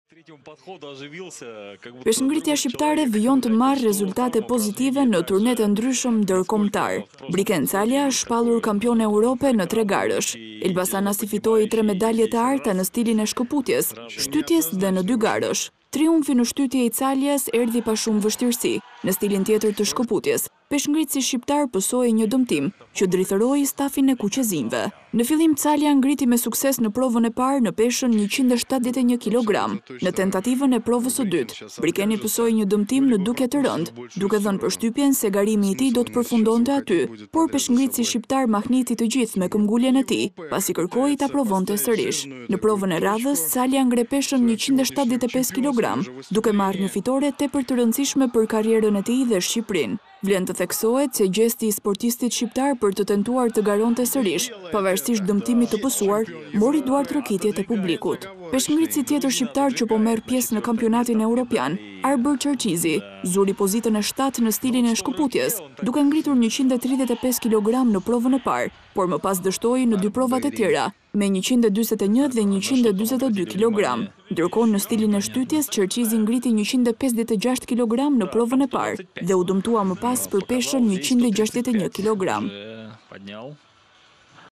Ora News - Briken Calja "sfumon" skandalin, fiton tre medalje ari në Europianin e Peshëngritjes
Peshëngritësi shqiptarë, Briken Calja ka vendosur diktatin në Europianin e Peshëngritjes, me 28 vjeçarin që triumfoj në të gjitha disiplinat duke rrëmbyer tre medalje ari.

Në Europianin e pëshëngritjes që po zhvillohet në Rumani, Calja realizoi të tre provat e tij në stilin e shkëputjes duke  ngritur fillimisht peshën 141 kilogram, me shumë lehtësi.

Në provën e dytë, 28 vjeçari ngriti 143 kilogram për ta mbyllur më pas me peshën 146 kilogram dhe duke "rrëmbyer" medaljen e artë në këtë stil.